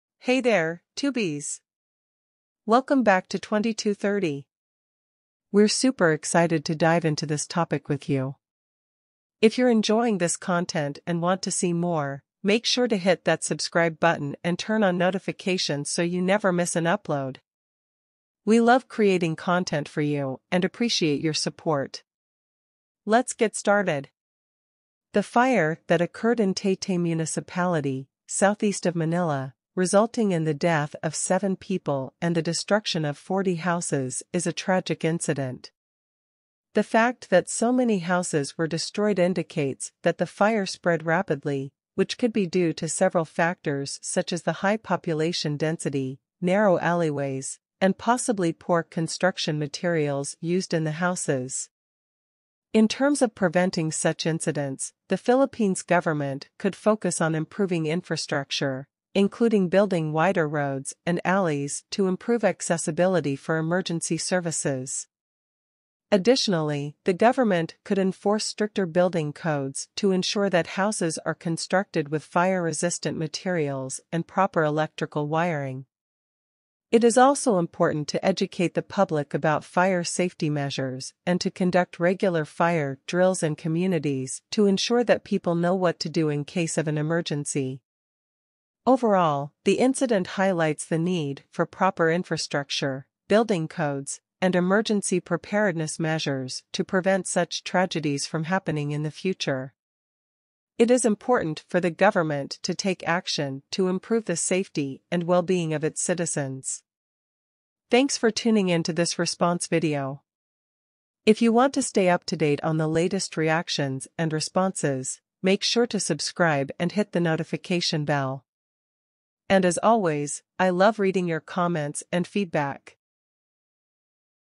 Seven dead, including two children, in fire near Philippines capital, Taytay
The fire that occurred on Saturday evening in Taytay municipality, southeast of Manila, Philippines, resulted in the death of seven people, including two children. The fire destroyed 40 houses, leaving at least 60 families homeless. The victims were caught in the flames, and some of them lived in a house accessible only through a long, narrow alley. They were unable to escape and were found hugging each other. The fire took around two hours to put out, and investigators suspect faulty wiring inside one of the houses as the possible cause. At least one person was injured by falling debris. It is a tragic incident, and my condolences go out to the families affected by this disaster.